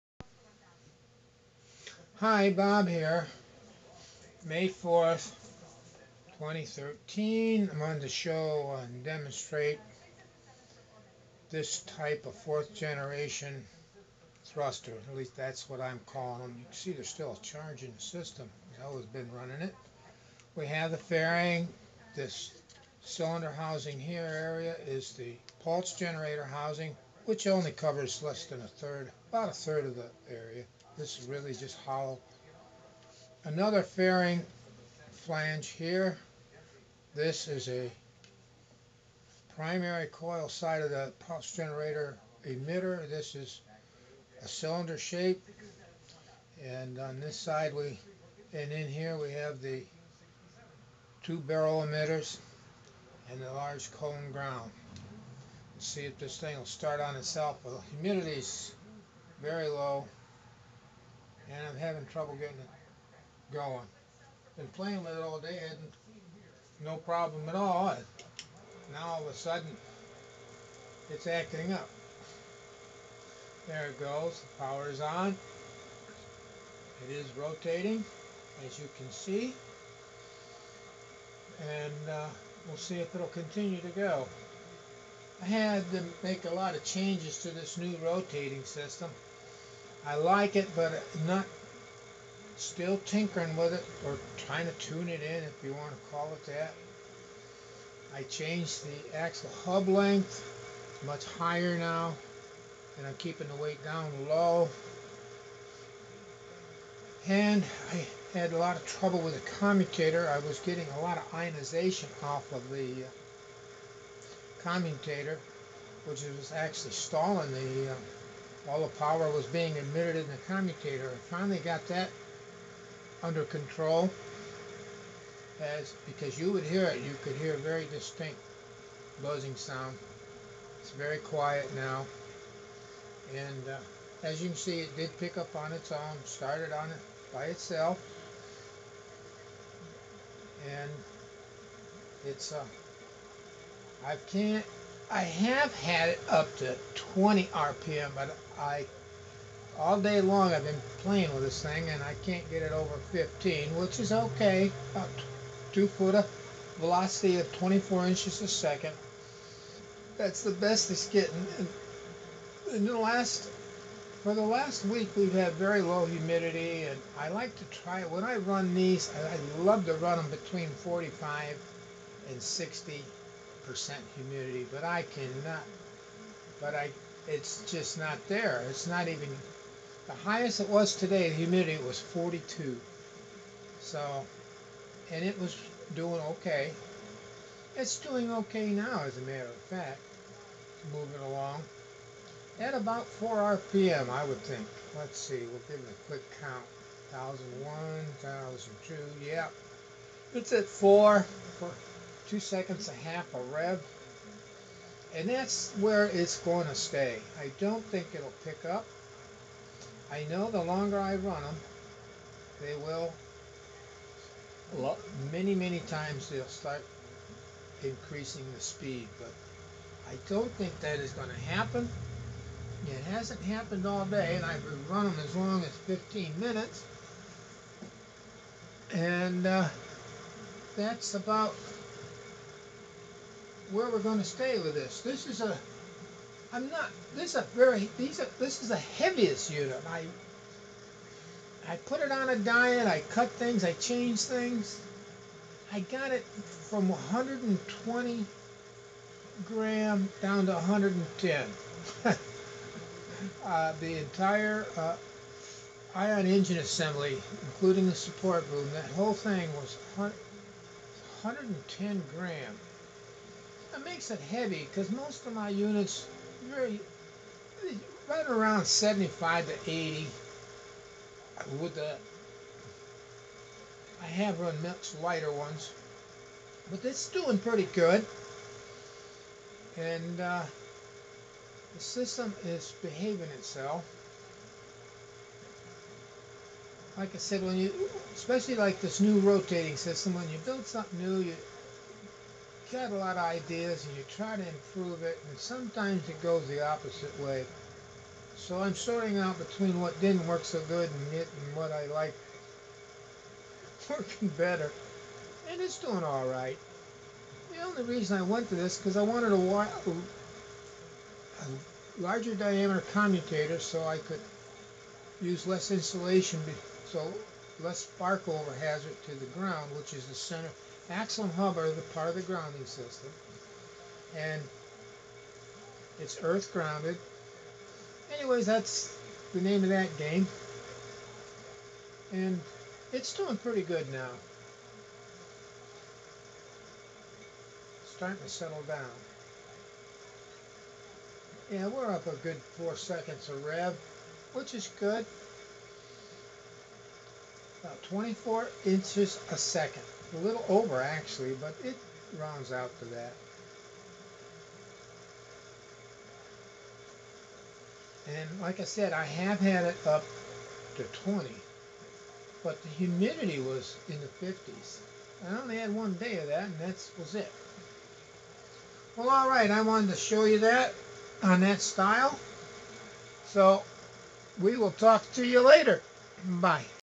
LARGE ION ENGINE
This ion engine is using two barrel type emitters and a large cylinder emitter. The pulse generator is a bifilar type located in the load carrying fairing. This is a heavier engine but performs well for its' size and weight. This unit does not exist any more as with all the other earlier engines. I only keep about five of my latest designs around. I keep videos of them for research reasons and to see if I'm actually improving their performance. Even though the weight of the units has increased, the velocity is at an average between 24 inches to 30 inches per second. The first units were between12 and 15 inches per second.The highest velocity I have ever achieved is 48 inches per second. My goal is 60 inches per second. I power all of the ion engines with a 200,000 volt static generator.  BOB